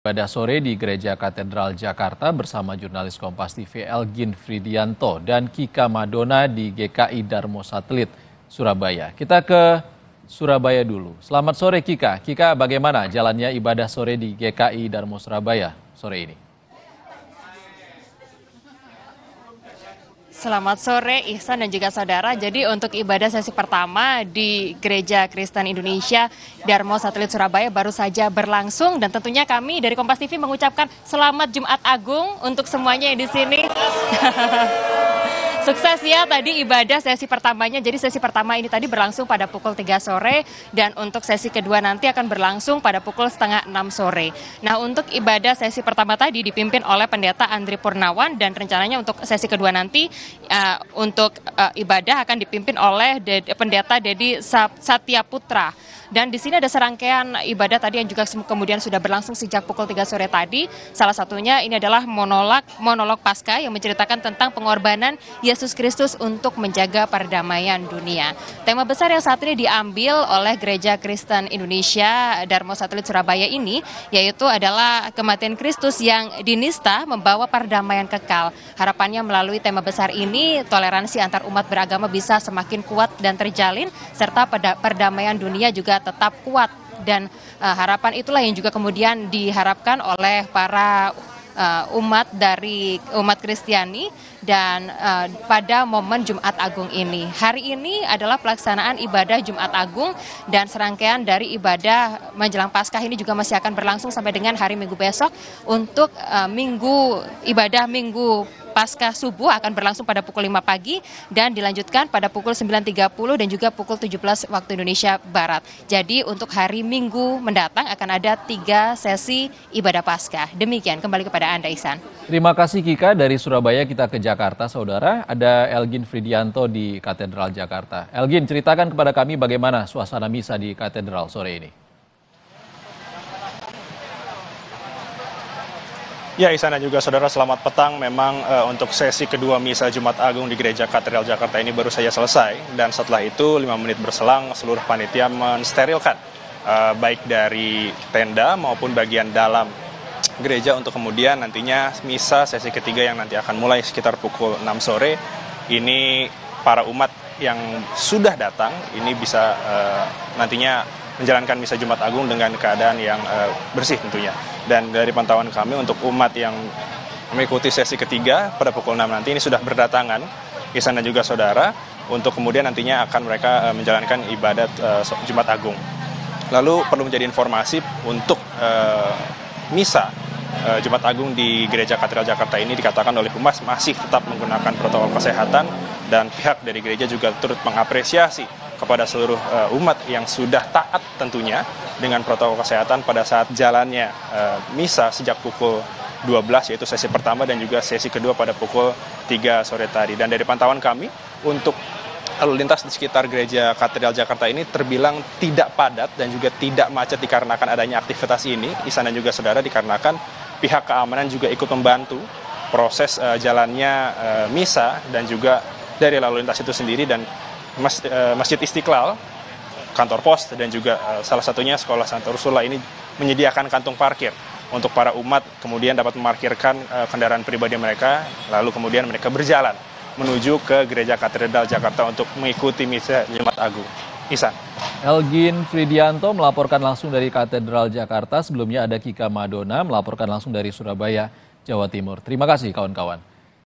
Suasana Jelang Ibadah Jumat Agung Sesi 3 Di Surabaya dan Jakarta
MAKASSAR, KOMPAS.TV -